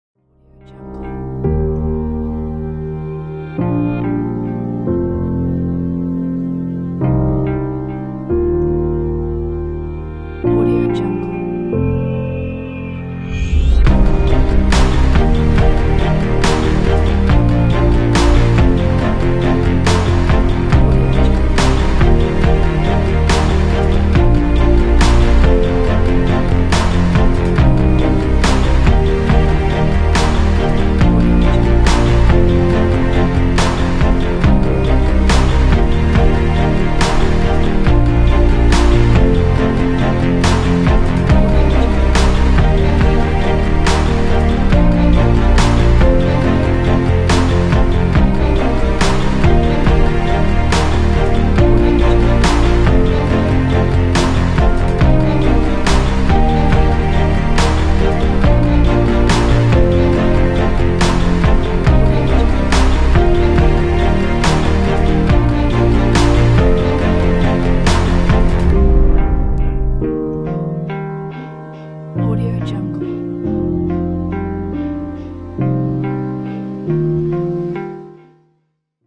20 Simple Titles - After Effects Project Files | VideoHive 16420189
20 Simple Titles 20 Titles 1080p and 4K 3840x2160 resolutions Easily to customize with the Controller: Scale, Color and Duration Universal Expressions Compatible with After Effects CS5 82... (titles) Read more